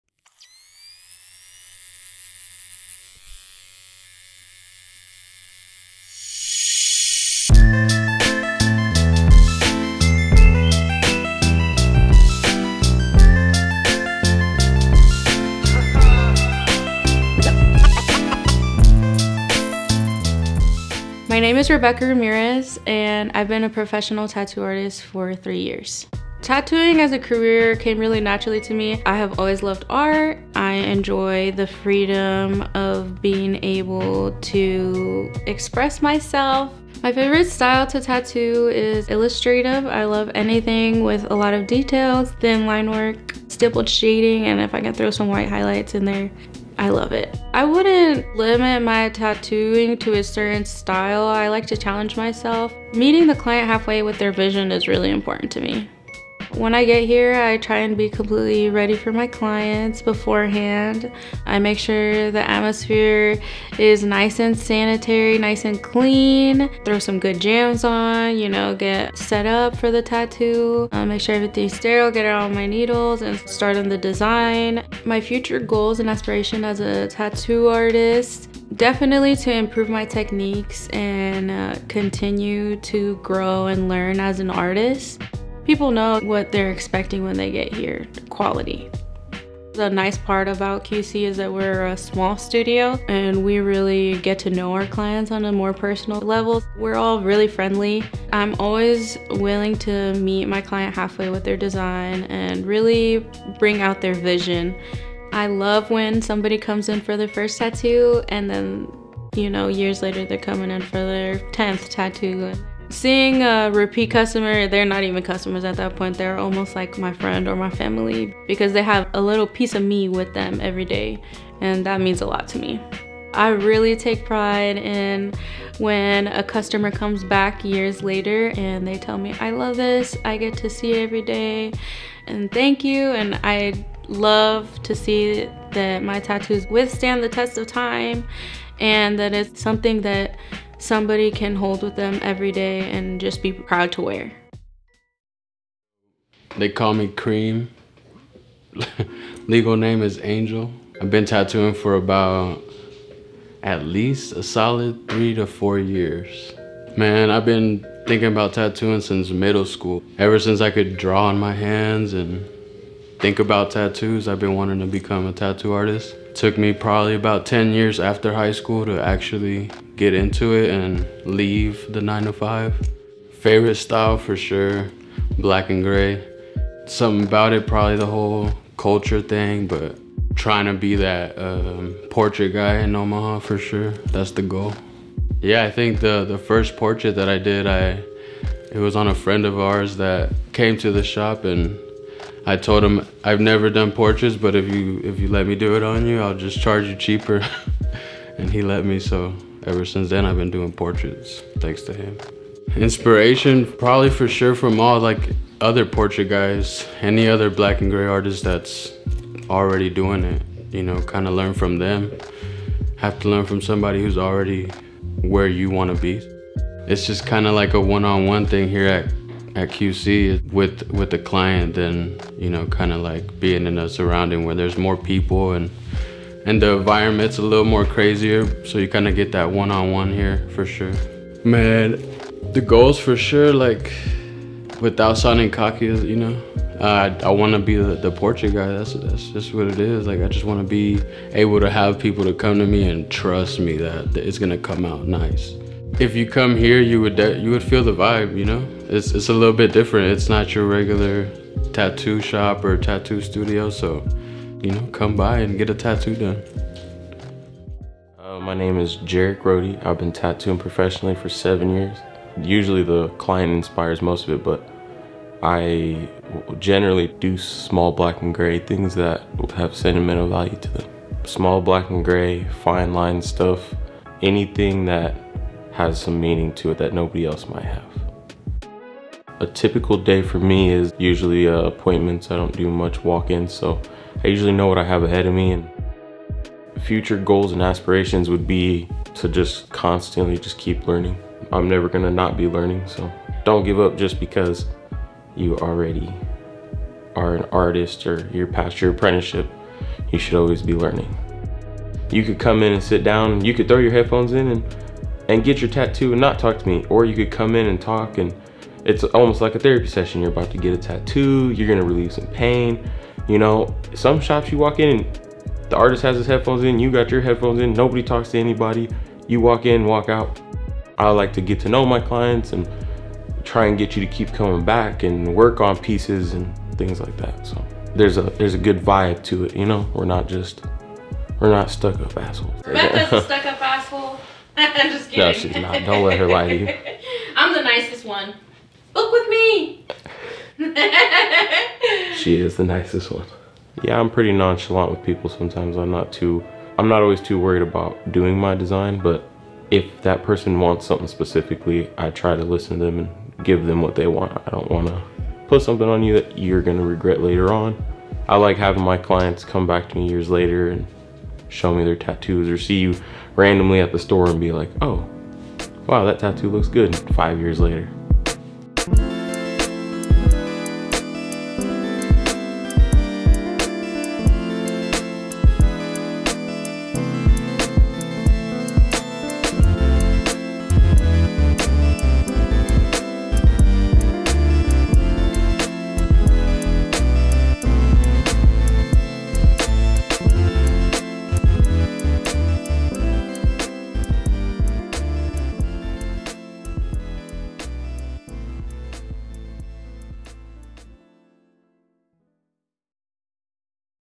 Fly Over This E01: QC Tattoo Studio
In this very first episode of the series, we visit QC Tattoo Studio in Omaha, Nebraska. Three local tattoo artists share about their work and experience in the industry.

Video was filmed, directed, and edited by Keegan Kalisch using the Original BMPCC with Meike cine lenses. Edited and graded in DaVinci Resolve.